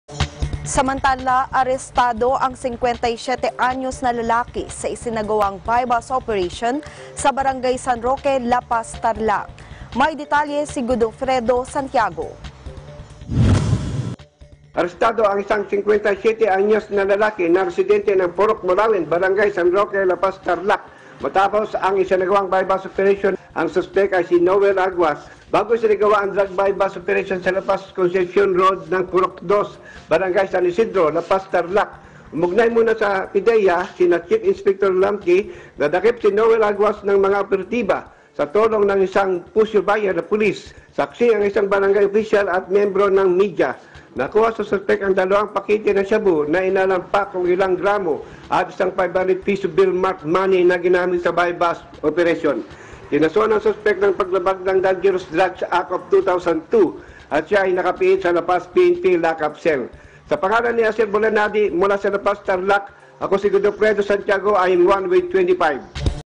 57-anyos na lalaki, arestado sa buy-bust operation sa La Paz , Tarlac
Arestado ang singkwetay siete anyos na lalaki sa isinagawang buy-bust operation sa Brgy. San Ruque, La Paz , Tarlac. (Godofredo Santiago, Eagle News Correspondent)